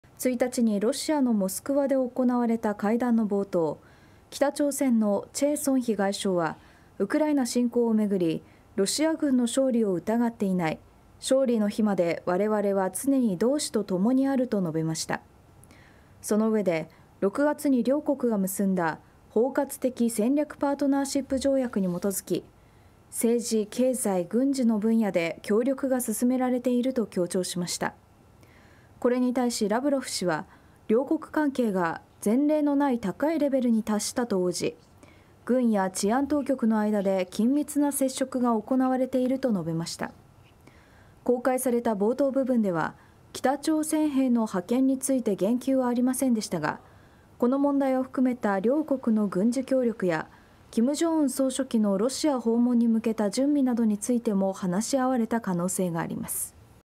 北朝鮮外相「ロシアの勝利まで同志と共にある」ロシア外相と会談、軍事協力など協議か｜TBS NEWS DIG
北朝鮮の兵士のロシア派遣をめぐり欧米や韓国などが警戒を強める中、北朝鮮の崔善姫外相がロシアのラブロフ外相と会談し、「ロシアが勝利するまで同志と共にある」と強調しました。

ロシアを訪問中の北朝鮮の崔善姫外相は1日、ラブロフ外相とモスクワで会談しました。崔氏は会談冒頭で、ウクライナ侵攻をめぐり、「ロシア軍の勝利を疑っていない。勝利の日までわれわれは常に同志と共にある」と述べました。

そのうえで、今年6月にプーチン大統領と金正恩総書記が署名した包括的戦略パートナーシップ条約に基づいて「政治、経済、軍事の分野で協力が進められている」と強調しました。

これに対し、ラブロフ氏は両国関係が「前例のない高いレベルに達した」と応じ、「軍や治安当局の間で緊密な接触が行われている」と述べました。

公開された冒頭部分では北朝鮮兵の派遣について言及はありませんでしたが、会談では、この問題を含めた両国の軍事協力や金正恩総書記の今後のロシア訪問に向けた準備などについても話し合われた可能性があります。